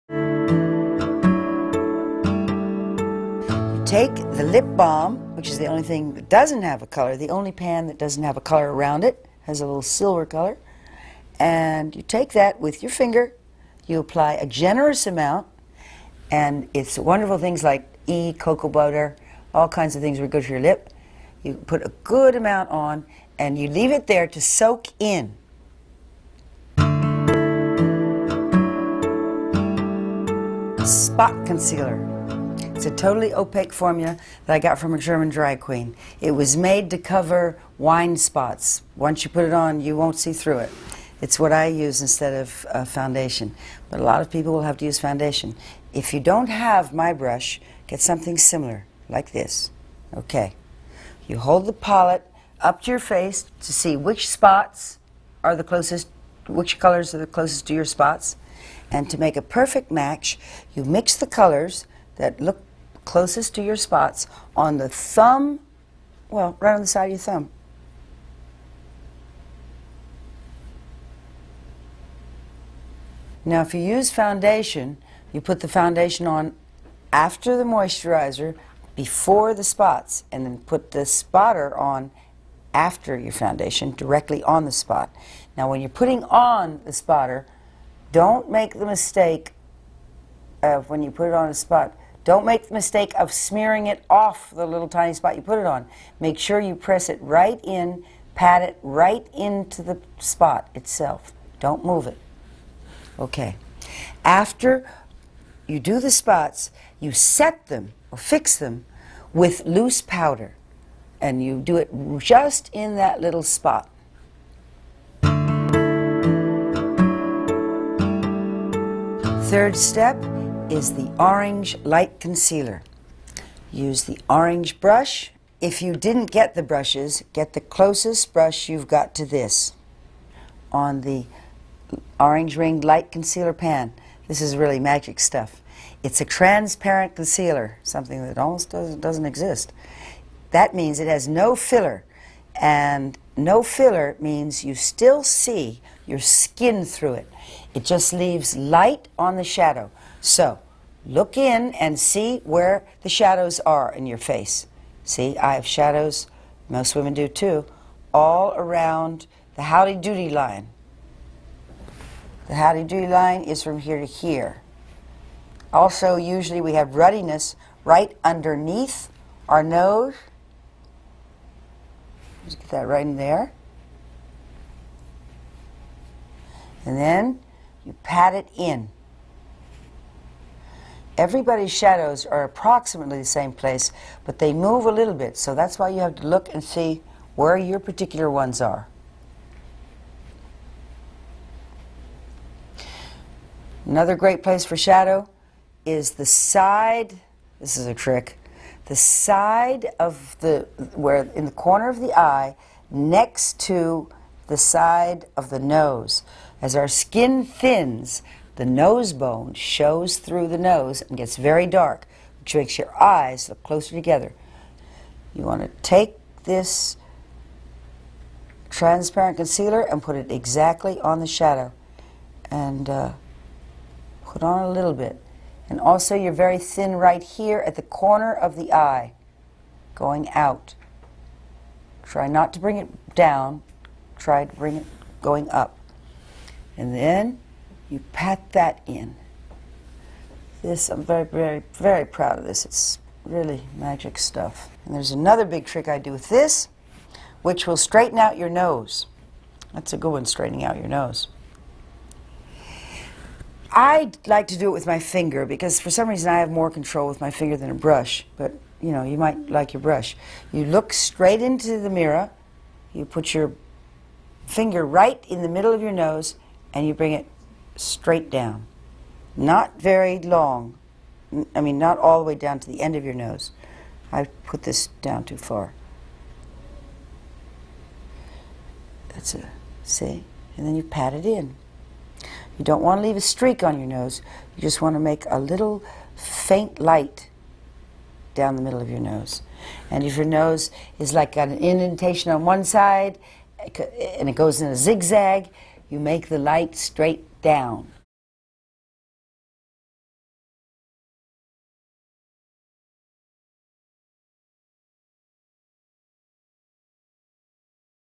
Lauren Hutton Make Up Tips!
When I returned to modeling at 46, I began researching and even mixing and blending my own products to enhance my features instead of masking them - with lightweight, sheer, shimmer - free formulas. The result is professional makeup, different from anything you've ever tried.